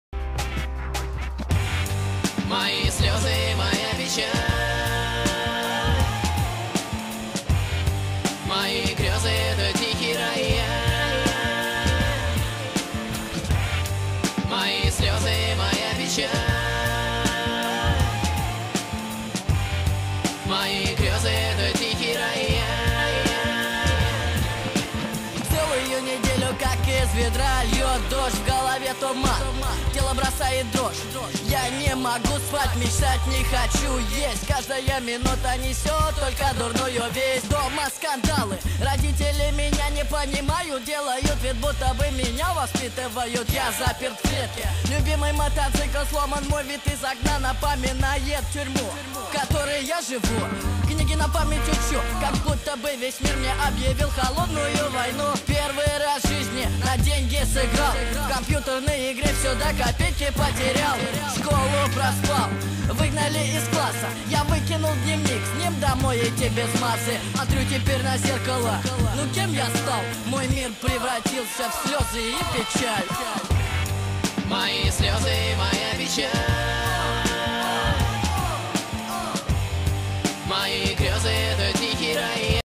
Слёзы, печаль, Спи спокойно , Децл
Децл ушел из жизни в 35 лет.
не являюсь большим поклонником рэп исполниnелей кроме "Грот" , однако Кирилл звучал в моей юности. Прощай